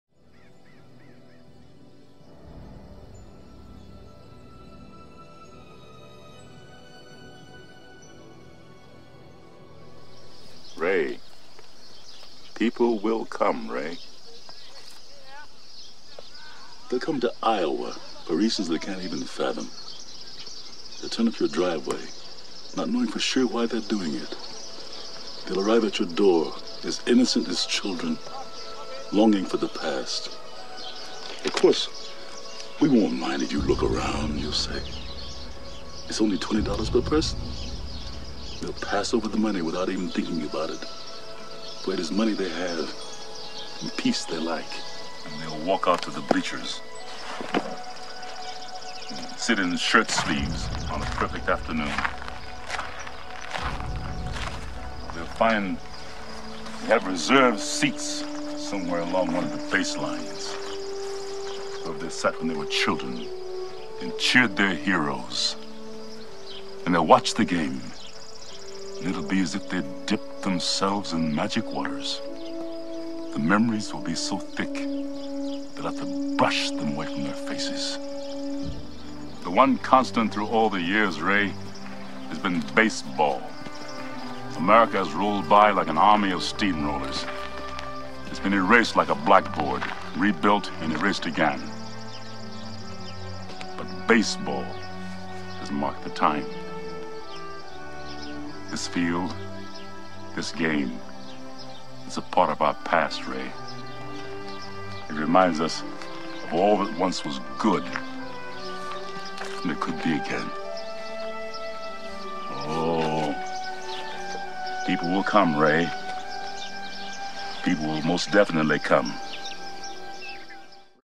Field of Dreams Game 2022 - cubs vs reds
Field of dreams game 2022 Chicago cubs vs Cincinnati reds Dyersville, is

Hastily filmed and edited entirely on iPhone